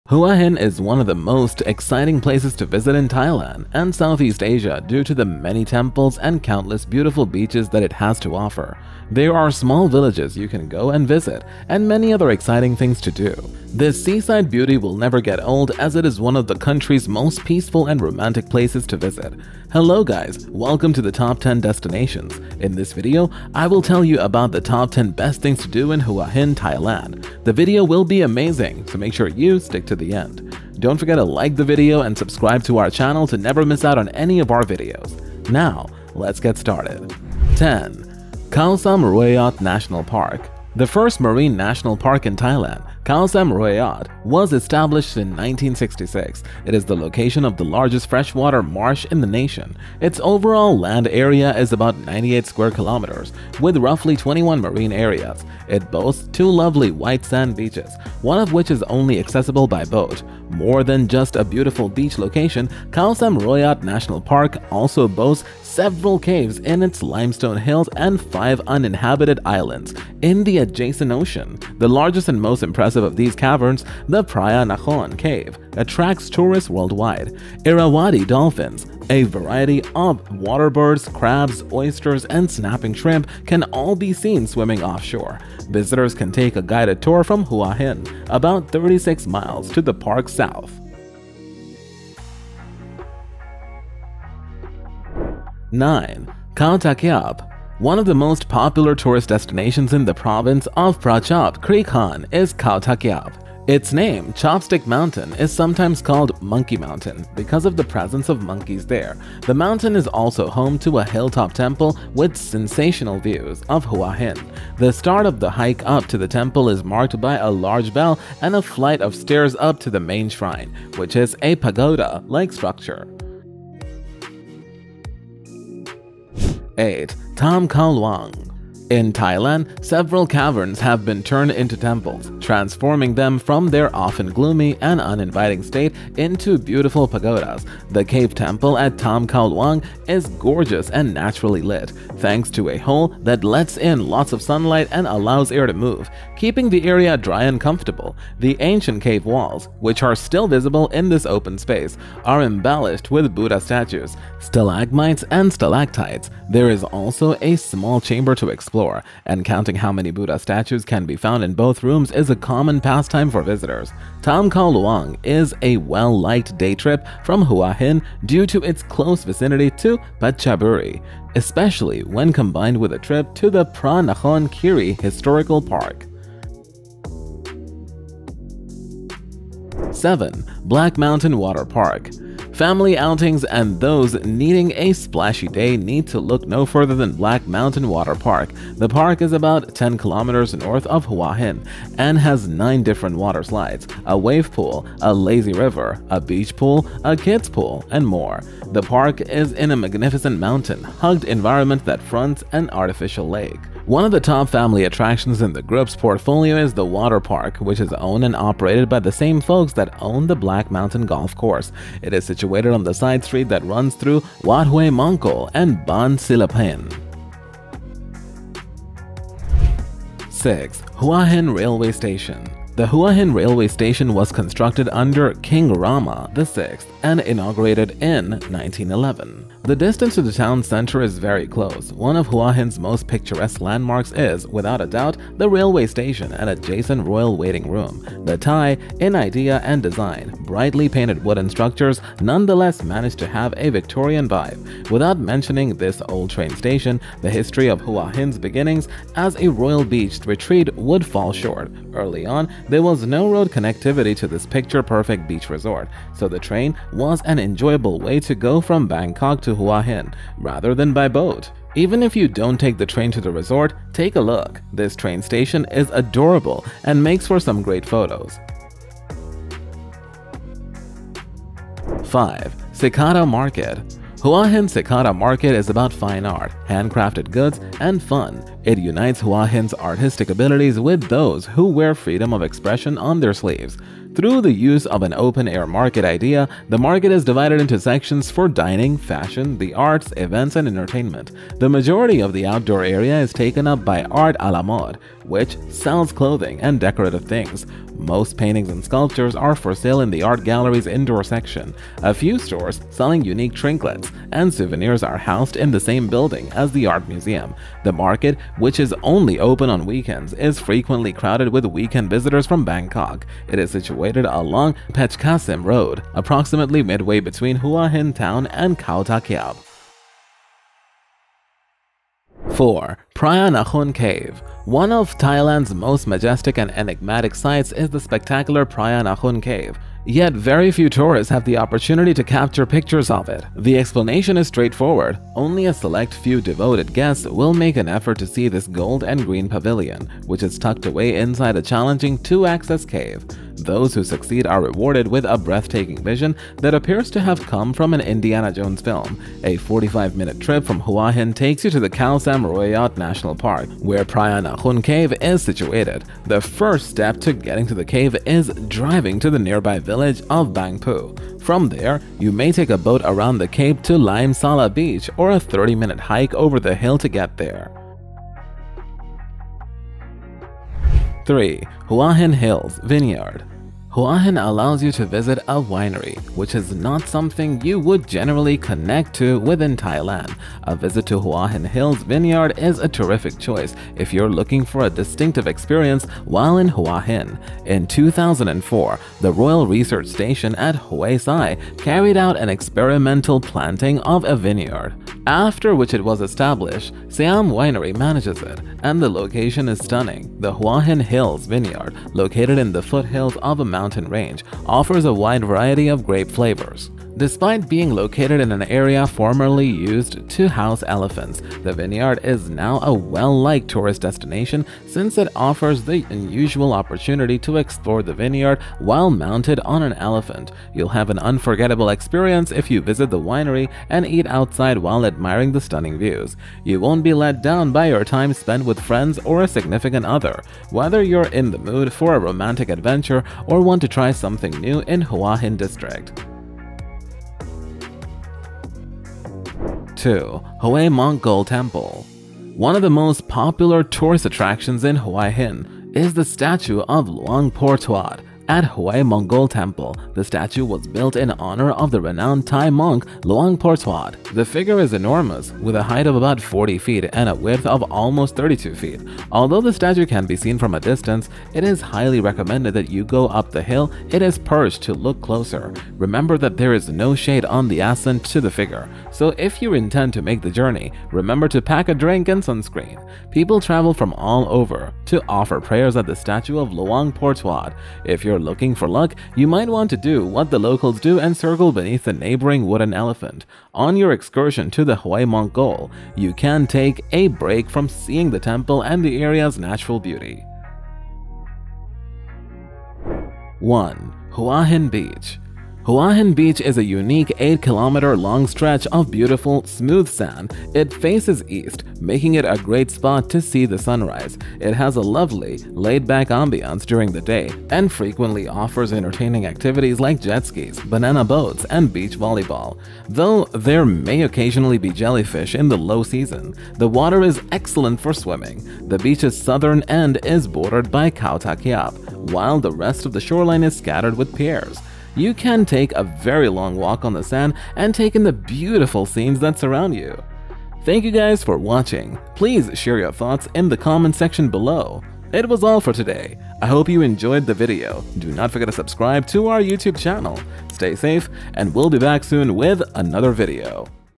Top 10 Things To Do in Hua Hin
Looking for the best things to do in Hua Hin? Look no further! In this video, I've listed the top 10 places to visit in Hua Hin. From the stunning temples to the bustling night markets, there's something for everyone in Hua Hin.

So, if you're planning a trip to this amazing place, then make sure to check out these places!

I hope you like this video and find it helpful.

Chapters:

0:00 - Why Visit Hua Hin
0:36 - 10. Koh Sam Roi Yot National Park
1:34 - 9. Khao Takiap
2:06 - 8. Tham Khao Luang
2:58 - 7. Black Mountain Water Park
3:38 - 6. Hua Hin Railway Station
4:30 - 5. Cicada Market
5:21 - 4. Phraya Nakhon Cave
6:09 - 3. Hua Hin Hills Vineyard
7:18 - 2. Huay Mongkol Temple
8:11 - 1. Hua Hin Beach